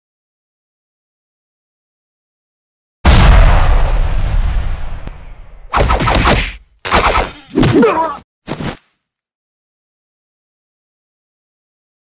RHG 5 beast vs sam
mi quinto RHG y es contra sam lamento lo cortos que son mis RHG pero nno he tenido mucho tiempo solo espero que sea de su agrado